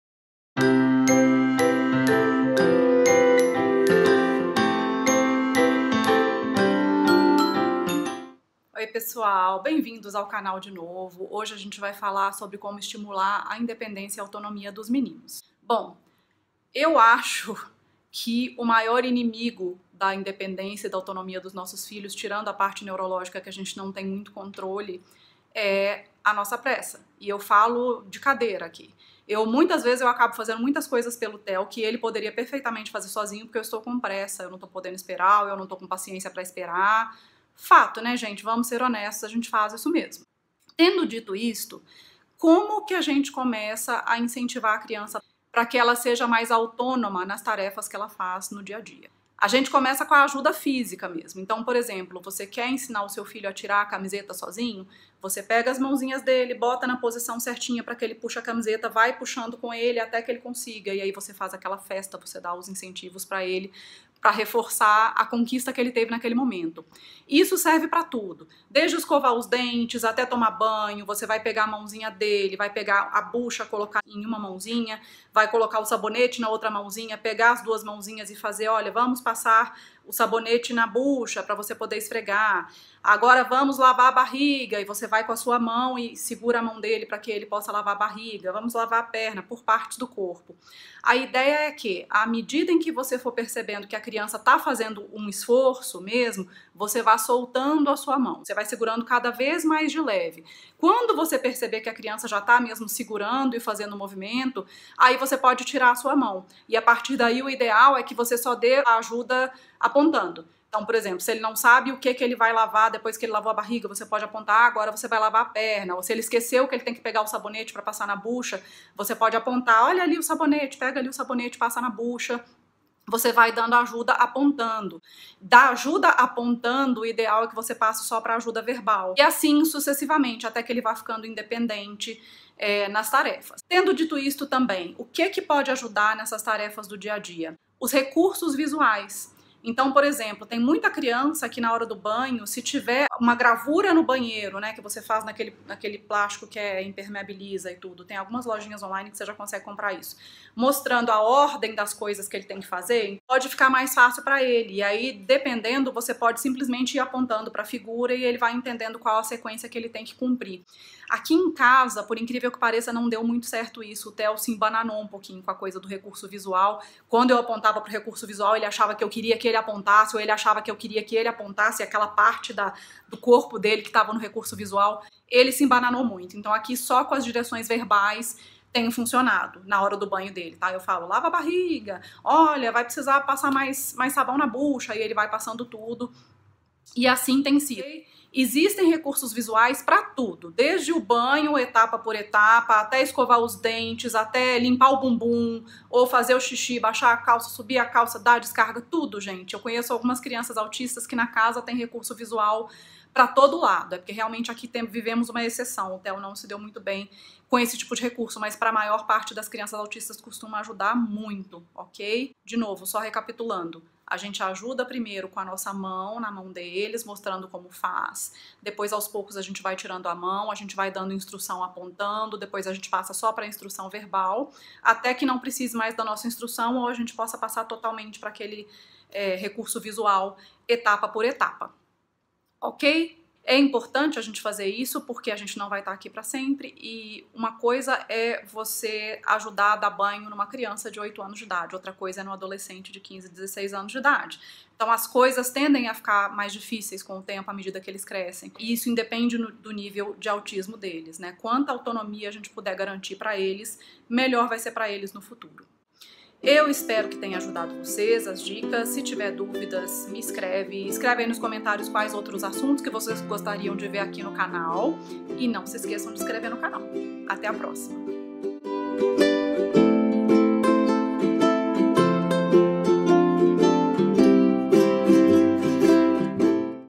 COMO ESTIMULAR INDEPENDÊNCIA E AUTONOMIA EM CRIANÇAS AUTISTAS
A pressa e a correria do dia a dia, muitas vezes, nos levam a fazer as coisas pelos nossos filhos. E isso acaba atrapalhando a aquisição de habilidades essenciais para desempenhar atividades diárias, como calçar os sapatos, tomar banho ou escovar os dentes. Neste vídeo, dou dicas do passo a passo que funcionou por aqui para estimular a autonomia em várias tarefas.